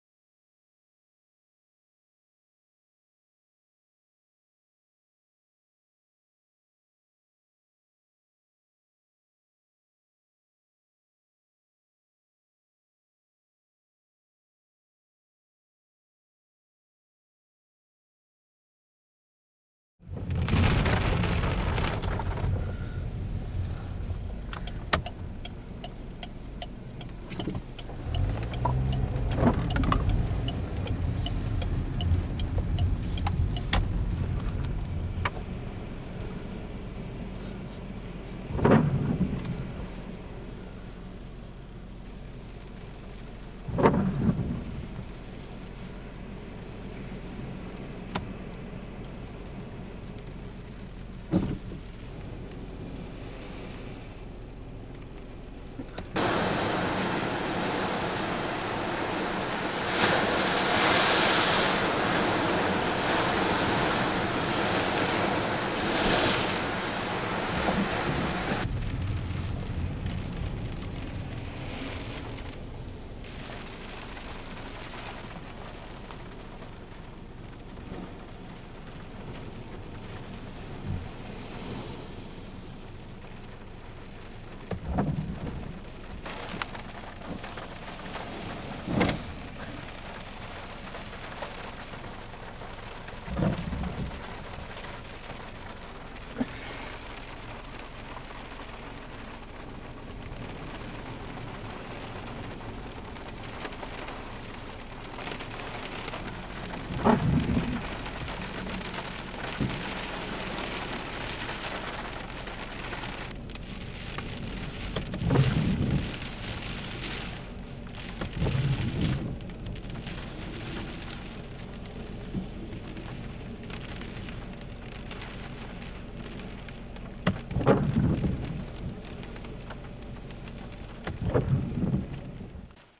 Lake Erie rocked by storm
Lake Erie was full of waves during a rain storm on Monday, September 28, 2009.